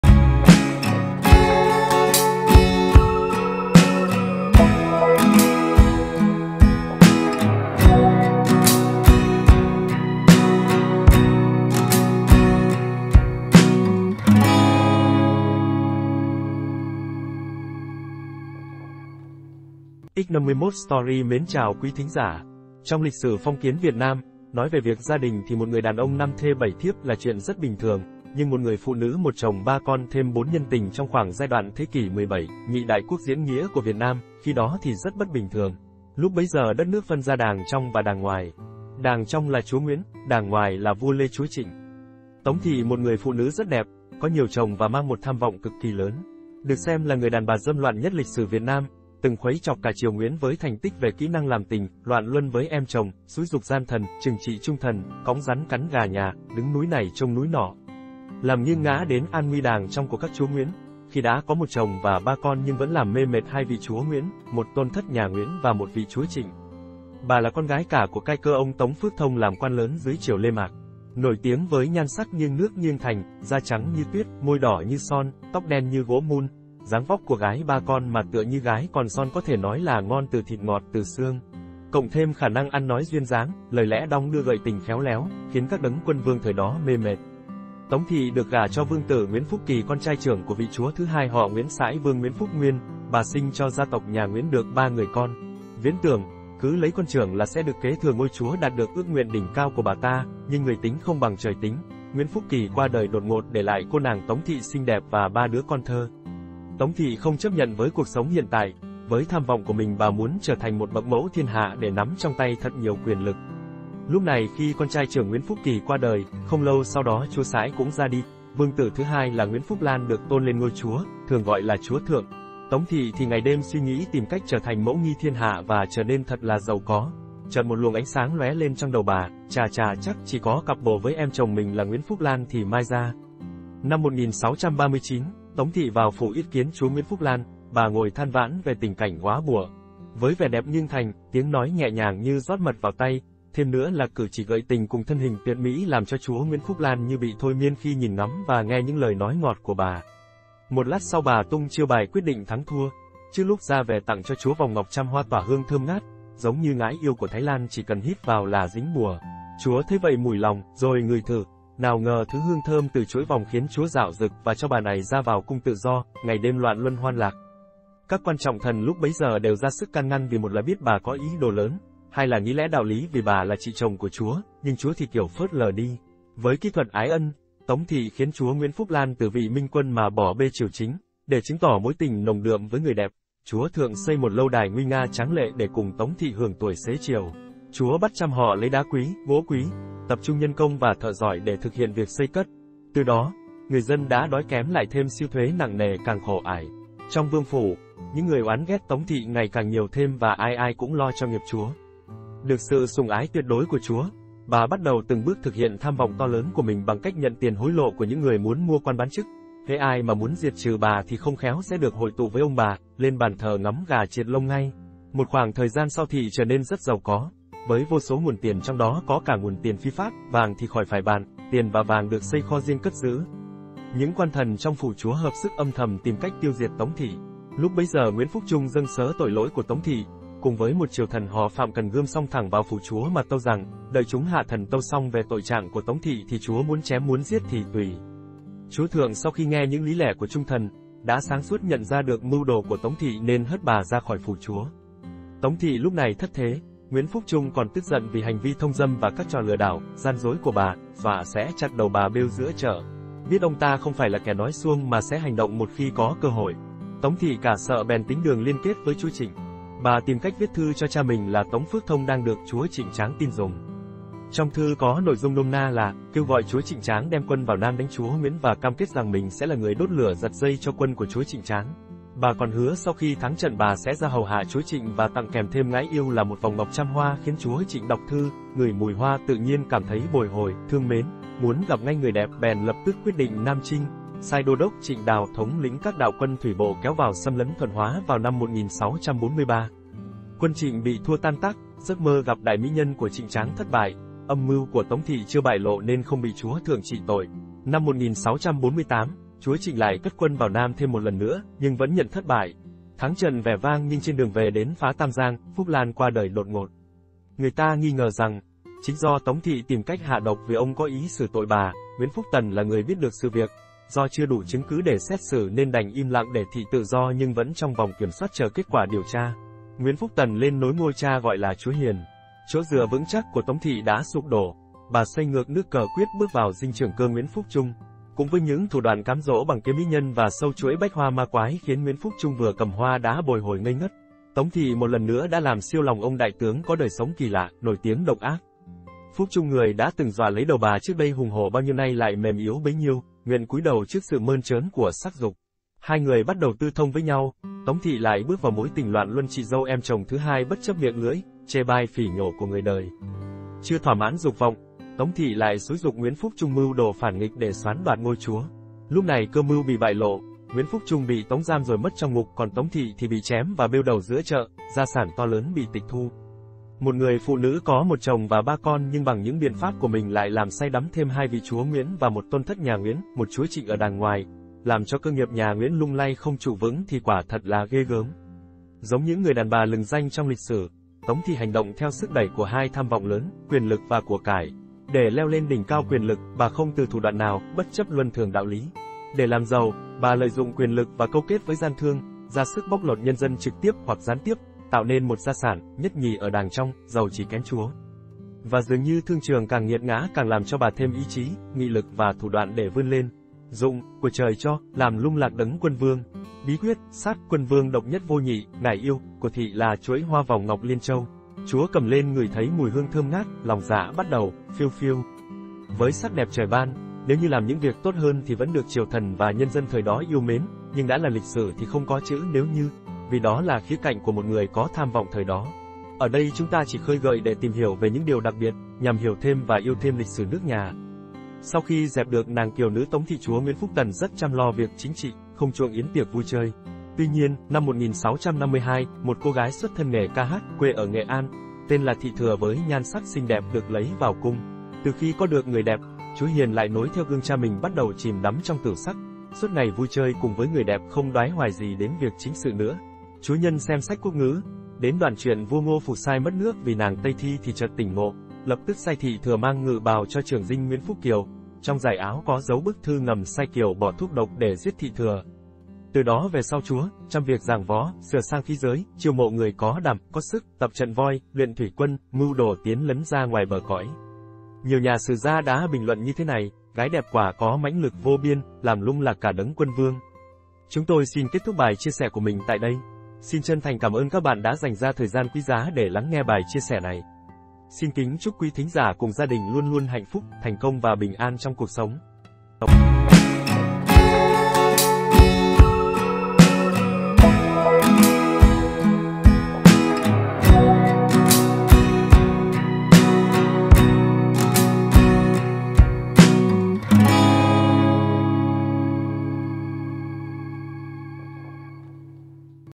BÍ ẨN NGƯỜI ĐÀN BÀ ĐẸP LÀM KHUYNH ĐẢO TRIỀU CHÍNH THỜI HAI NHÀ TRỊNH- NGUYỄN PHÂN TRANH
Trong lịch sử phong kiến Việt Nam, nói về việc gia đình thì một người đàn ông năm thê bảy thiếp là chuyện rất bình thường, nhưng một người phụ nữ một chồng ba con thêm bốn nhân tình trong khoảng giai đoạn thế kỷ 17 " nhị đại quốc diễn nghĩa của Việt Nam" khi đó thì rất bất bình thường. Lúc bấy giờ đất nước phân ra đàng trong và đàng ngoài. Đàng trong là Chúa Nguyễn, đàng ngoài là vua Lê – Chúa Trịnh.